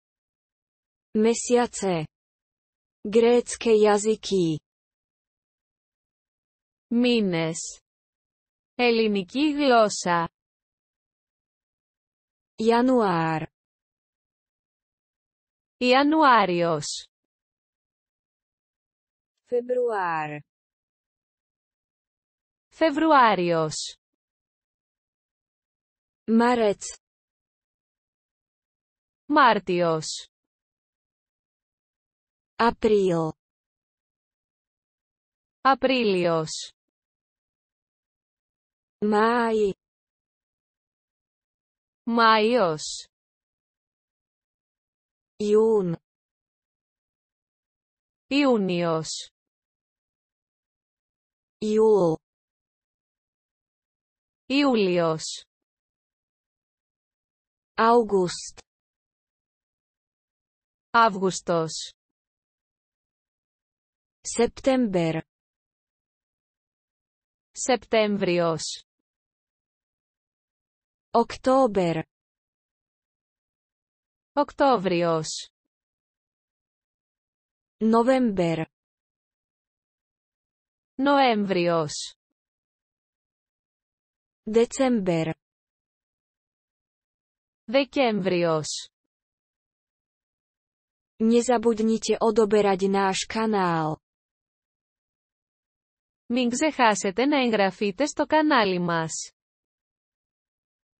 Mesiace (Grécke jazyky) (sk-el)
Fone TV
sk-el-22
Μήνες (Ελληνική γλώσσα)
Január - Ιανουάριος
Február - Φεβρουάριος
Marec - Μάρτιος
Apríl - Απρίλιος
Máj - Μάιος
Jún - Ιούνιος
Júl - Ιούλιος
August - Αύγουστος
September - Σεπτέμβριος
Október - Οκτώβριος
November - Νοέμβριος
December - Δεκέμβριος